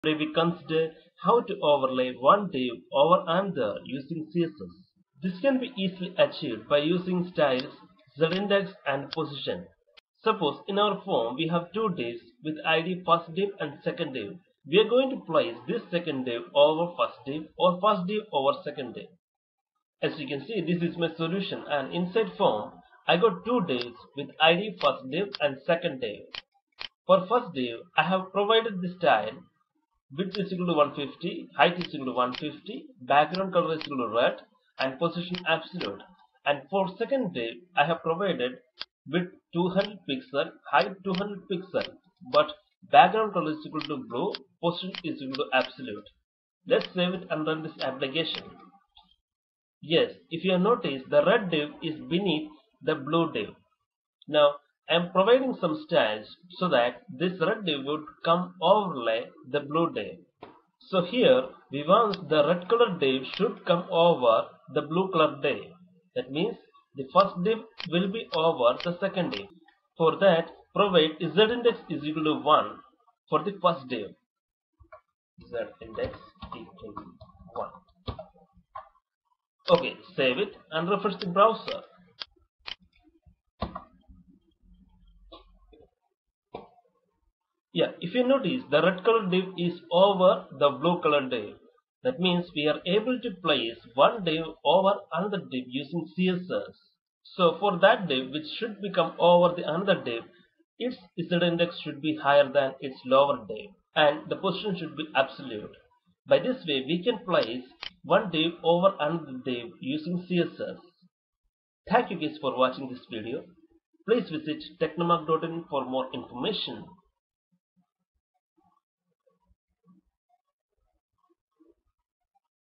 How To Overlay One Div Over Another Div Using CSS
for more information.

Complete steps is described in this video regarding How To Overlay One Div Over Another Div Using CSS